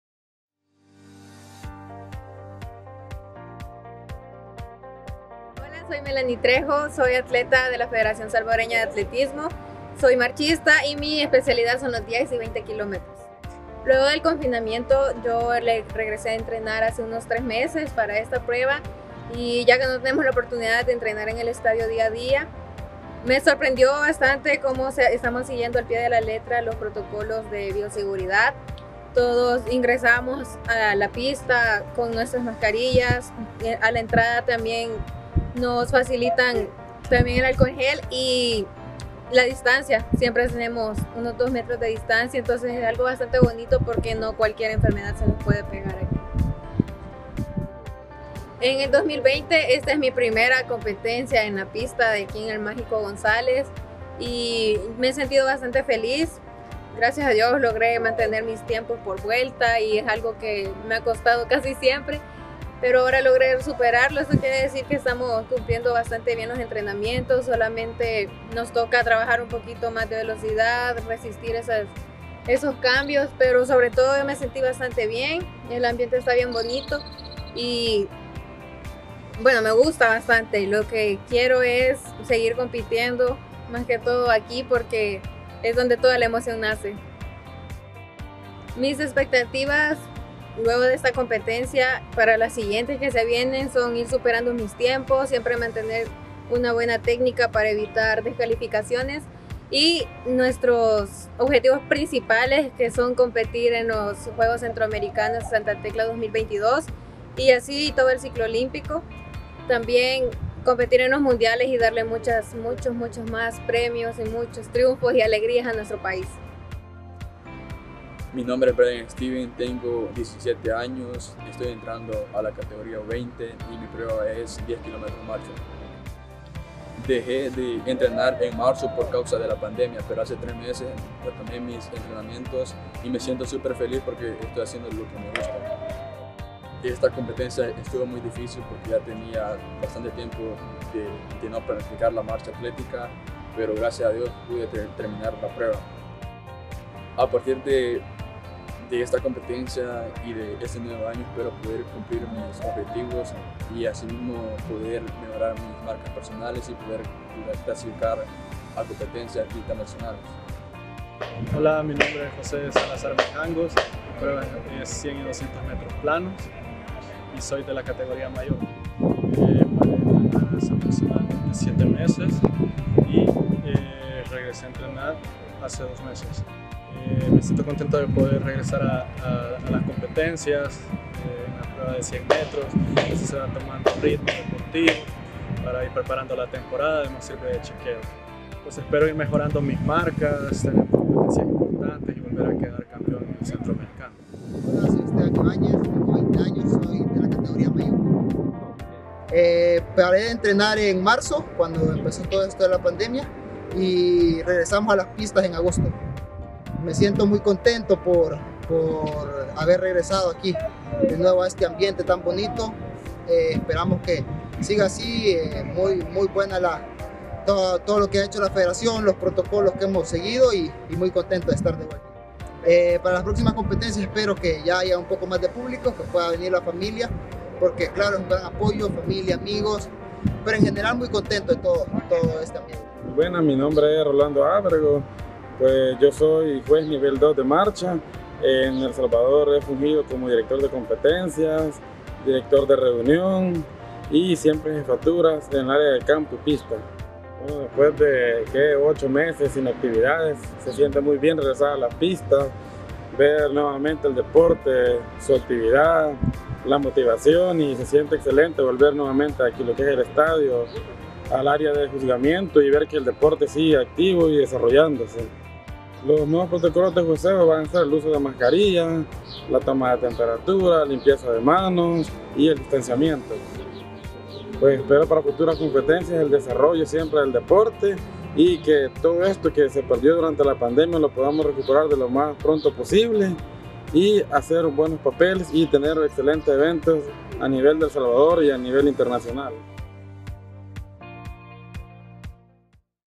28NOV2020 - Regreso a la pista - Declaraciones de los protagonistas
El pasado sábado 28 de noviembre se realizó la primera fecha del Campeonato Nacional de Marcha y el Torneo Regreso a la Pista, primeras dos competencias realizadas por la Federación Salvadoreña de Atletismo luego del confinamiento de casi 8 meses por la pandemia del COVID-19.